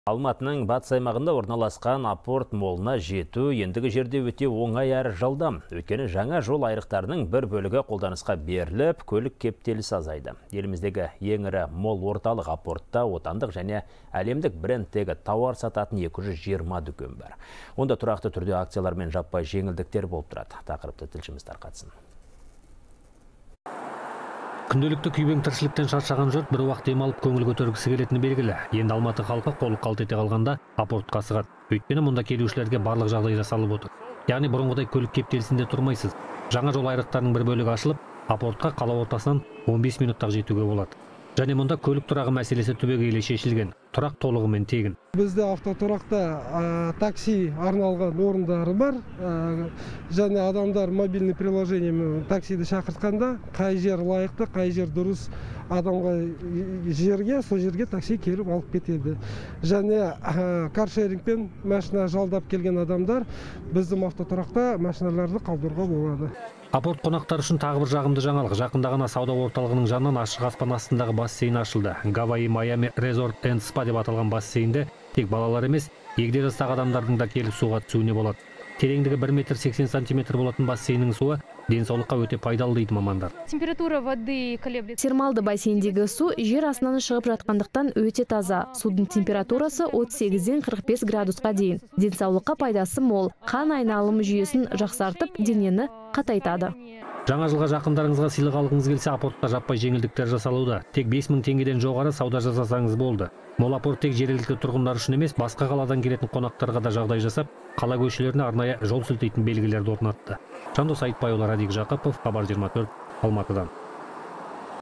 «Апорт» қонақтары үшін жағымды жаңалық!
Алматының батыс аймағында орналасқан «Апорт» молына жету ендігі жерде өте оңай әрі жылдам.